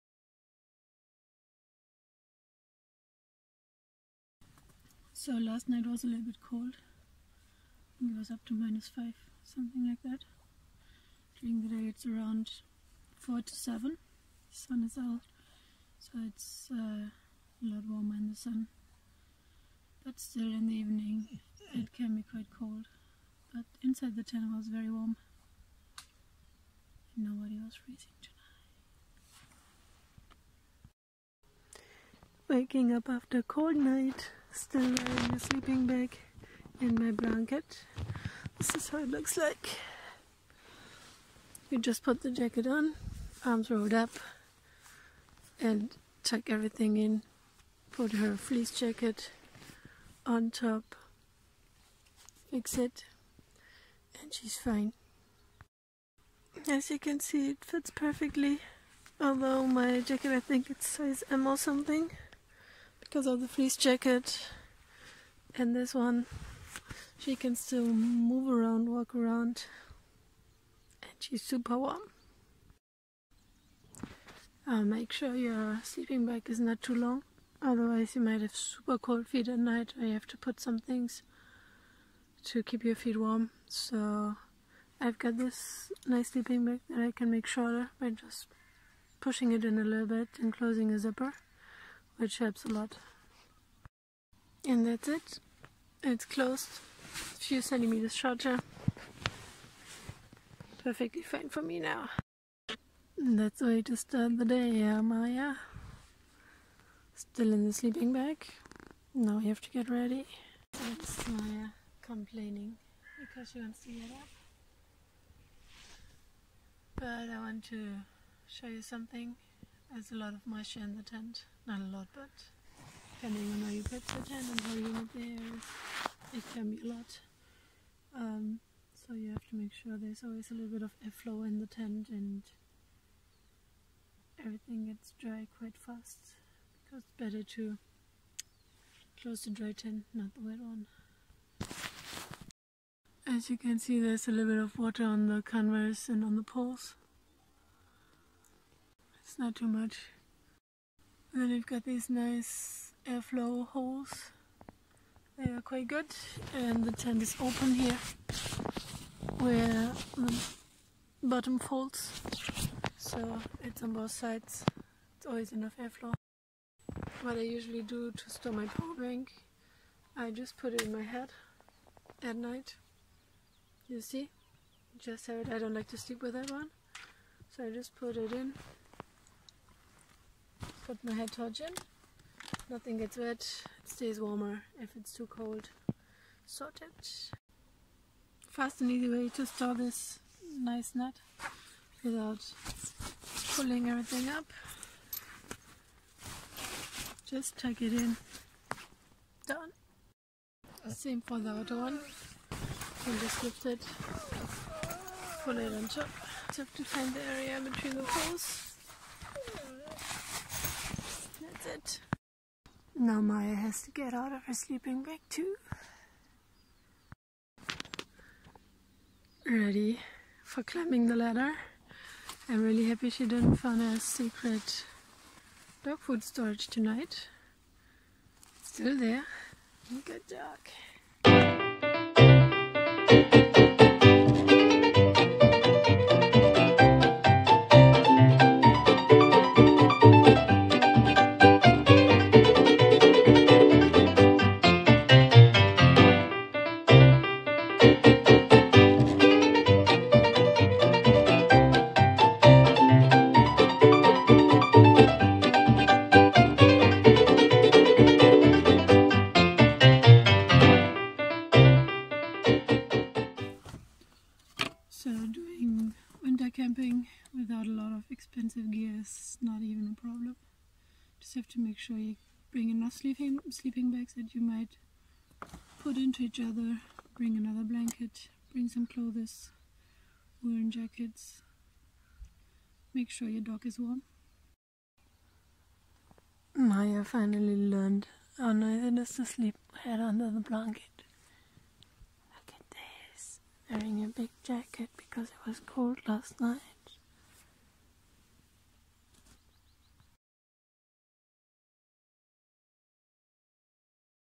Winter Camping Routine with a dog and rooftent. Dog Sleeping Bag for up to -25°C in a car or tent.
My camping routine with Maya. Improvised dog sleeping bag. No need for expensive gear. Use what you've got.

First try of climbing the ladder to the rooftent on my Hyundai Galloper.